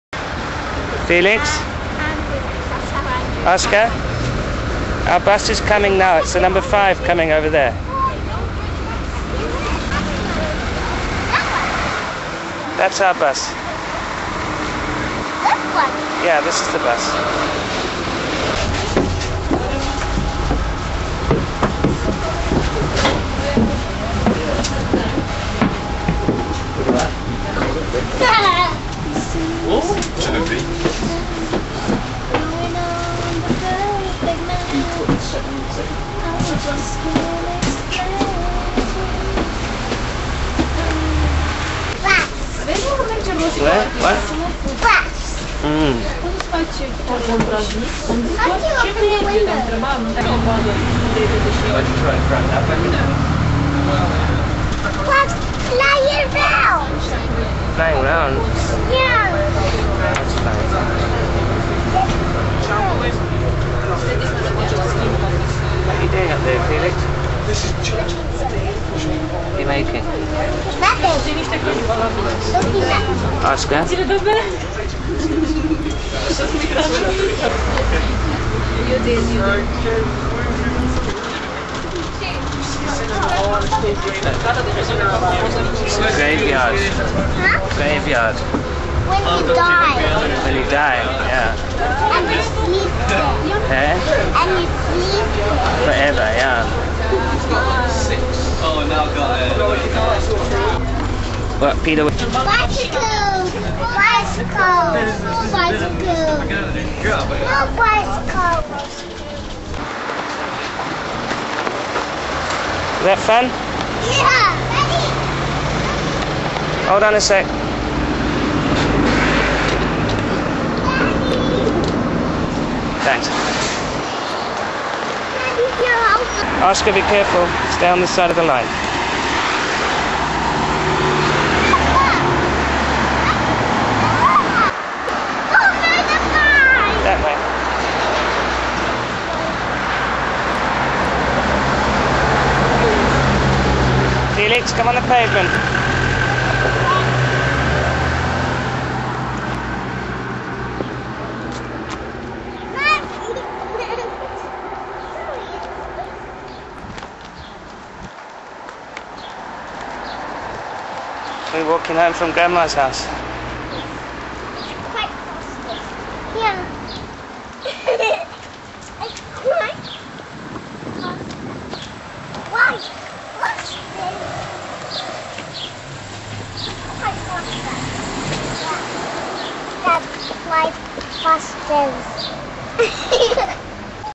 Bus
Although this should probably have been called School Bus since that's what the boys insist on calling it. Trip from Douglas to Grandma's house in Union Mills on the double decker and of course the boys like to sit on top and up front. If you can hear it, the two girls in front are talking in Manx.